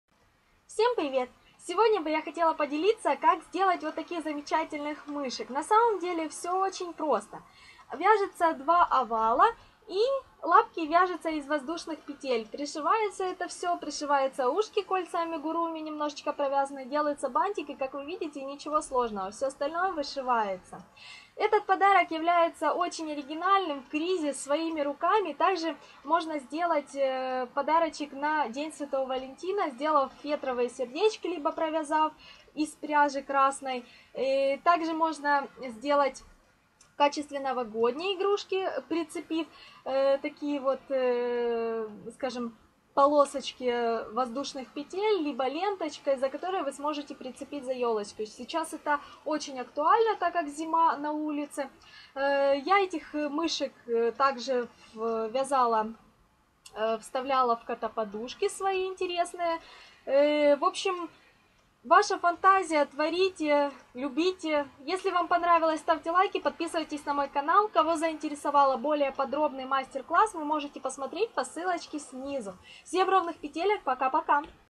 Амигуруми+вязание крючком мышки,как связать крючком игрушки амигуруми.
Ирина Загородняя#Игрушки амигуруми-краткое описание как связать мышь амигуруми,подписываемся на мой канал ,и вяжем вместе.Полное описание же принимаю заказы.